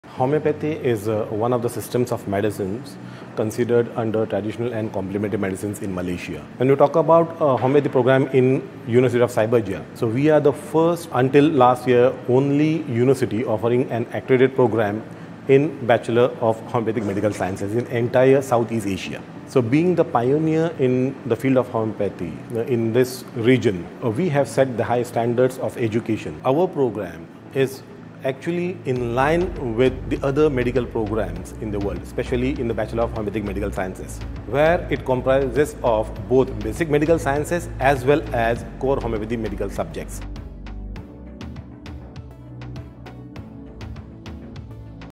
UoC's Homeopathy programme is the first and only in South East Asia
As a pioneer in the field of Homeopathy at the University level in South East Asia, Dr. Ibrahim, our Dean from the Faculty of Traditional and Complementary Medicine explains why this is significant.